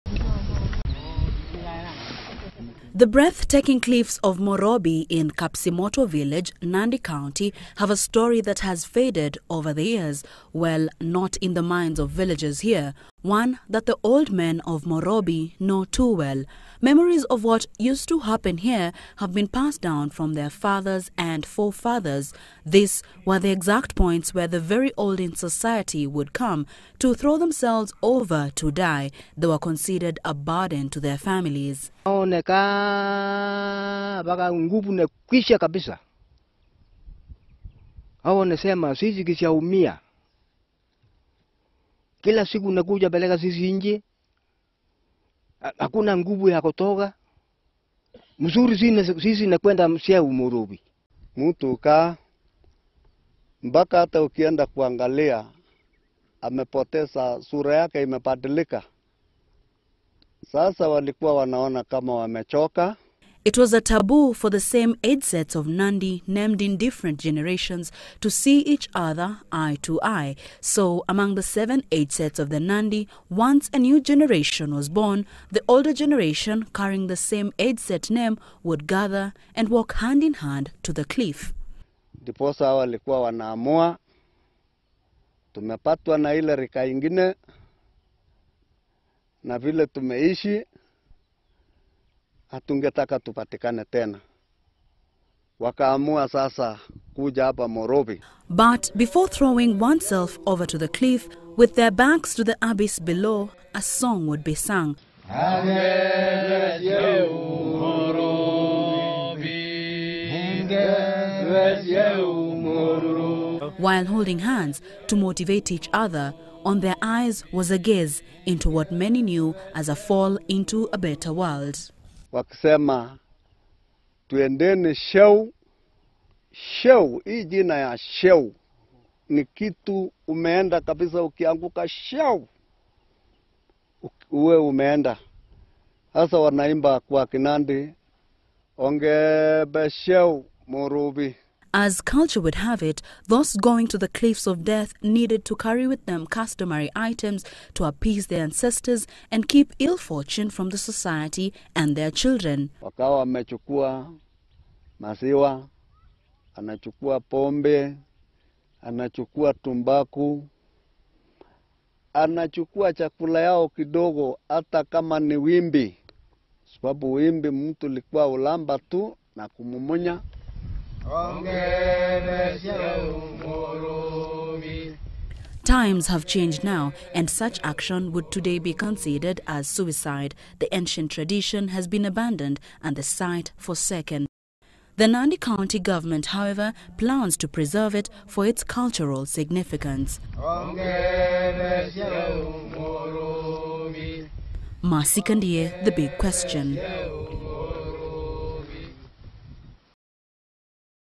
Morobi Cliffs in Nandi county hold dark ancient secrets
Masked by the breathtaking beauty of Morobi cliffs in Kapsimotow village, Nandi County are their dark history known only to locals.
Few recall an ancient practice that would see elderly men and women jump to their deaths in their journey to, well, the spirit world.